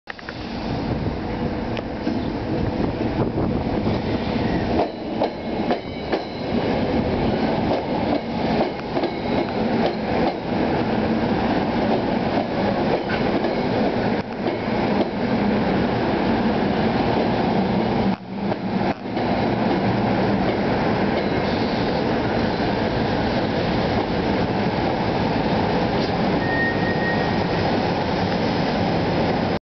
SEPTA Train
Arriving at West Trenton Station at 7:01 p.m. to pick up passengers before departing at 7:03 p.m. to Elwyn as Train #383 with GE Silverliner IV #234 leading & 163 bringing up the rear. I caught this consist earlier as express #4382 from Elwyn & Philadelphia.